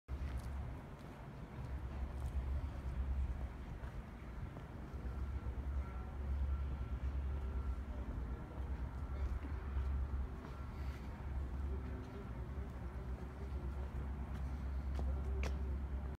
Walk Around of a Completed Painted Dorkpod Kit - DIY Vertical Electric Vehicle
beDorky and Tinkersmiths Makerspace brings you the Dorkpod DYI Open Source Vertical Vehicle Kits.  Help support our Makerspace with this project.